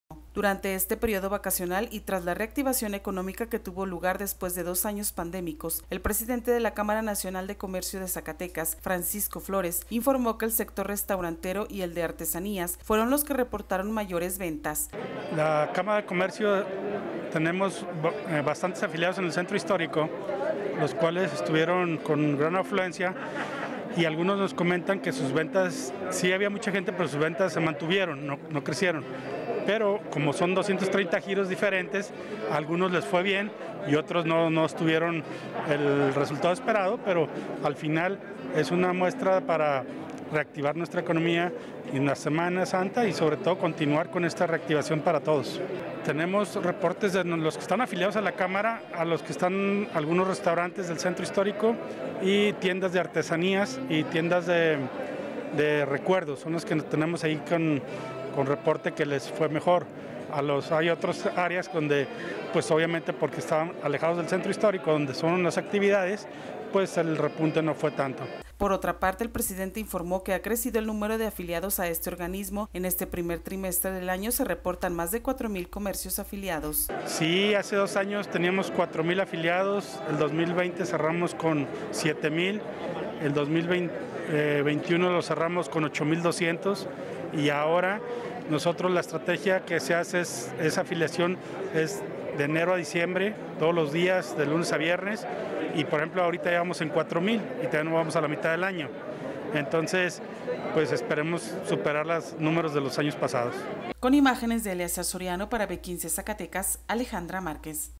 Comercios afiliados a la Canacozac reportan buenas ventas durante el periodo vacacional
Zacatecas, Zacatecas. – Durante este periodo vacacional y tras la reactivación económica que tuvo lugar después de dos años pandémicos, el presidente de la Cámara Nacional de Comercio de Zacatecas (Canacozac), Francisco Flores informó que el sector restaurantero y el de artesanías fueron los que reportaron mayores ventas.

Por otra parte, el presidente informó que ha crecido el número de afiliados a este organismo, en este primer trimestre del año se reportan más de 4 mil comercios afiliados.